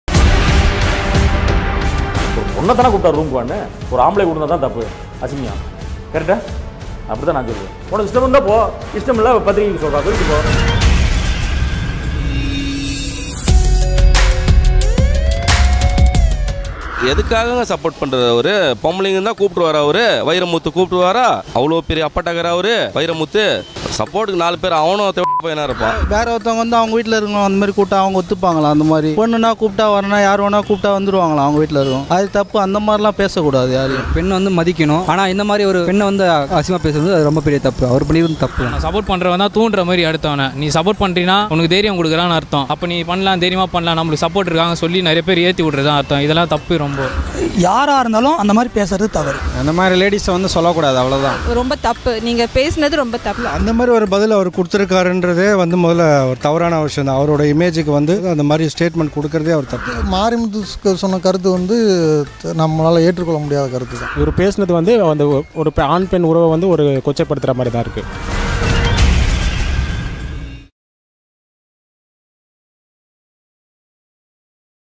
வைரமுத்துக்கு மாரிமுத்து சப்போர்ட் | Makkal Karuthu | Makkal Enna Soldranga
வைரமுத்துவின் தீவிர ரசிகரான மாரிமுத்து.
‘பரியேறும் பெருமாள்’, ‘கடைக்குட்டி சிங்கம்’
போன்ற படங்களில் நடித்தவர்.
வைரமுத்து ஒரு ஆண்.
அவருக்கு ஆர்மோன்ஸ் சுரக்கின்றன.
அதனால், ஒரு பெண்ணை கட்டிப்பிடித்து படுத்தால்
நன்றாக இருக்கும் என்று தோன்றி இருக்கிறது.